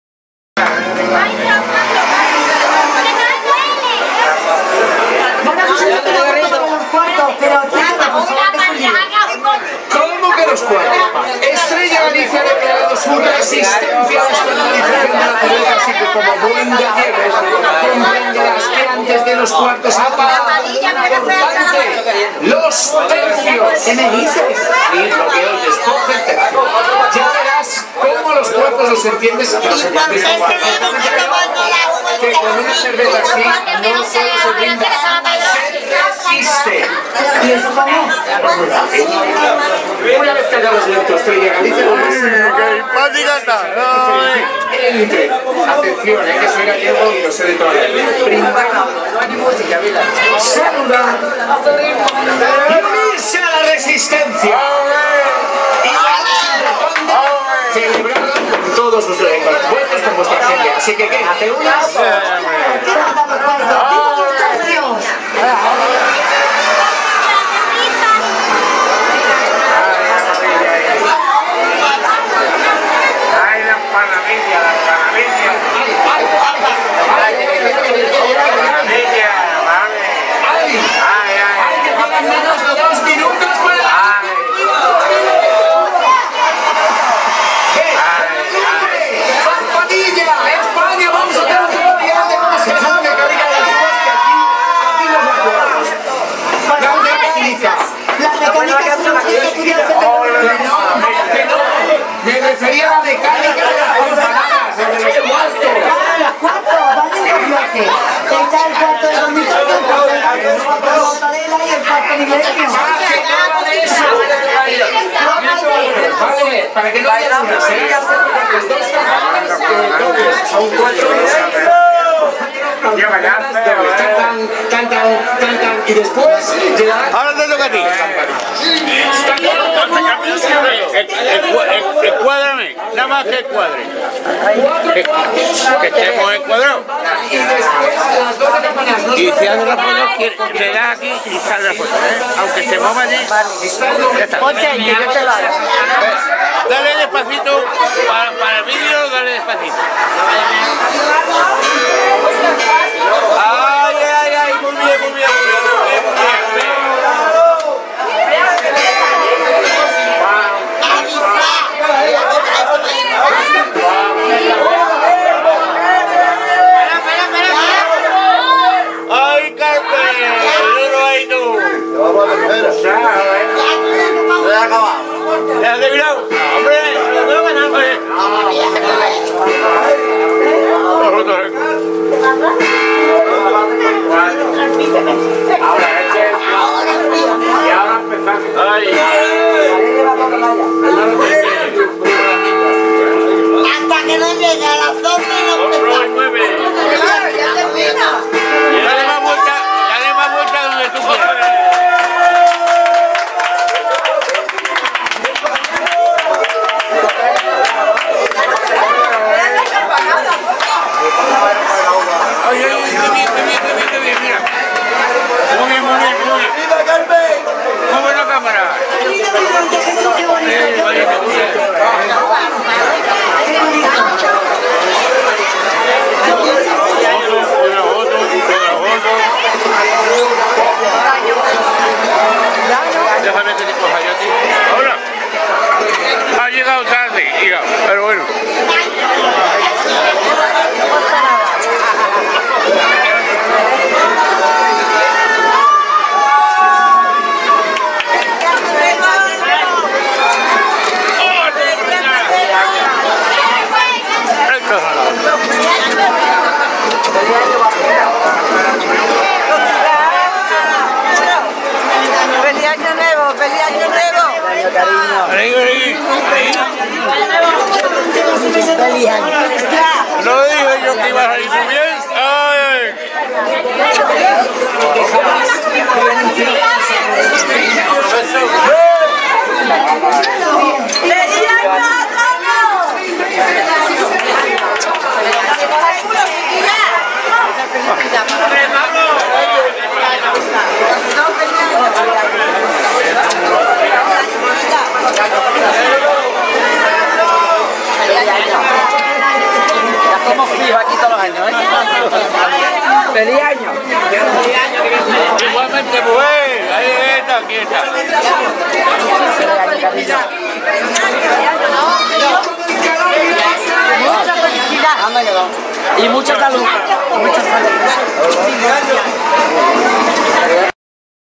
BENALUP-CASAS VIEJAS Feliz Año 2020 EL JARDÍN PROHIBIDO
Desde el Bar-Restaurante EL JARDIN PROHIBIDO os deseamos FELIZ AÑO 2020.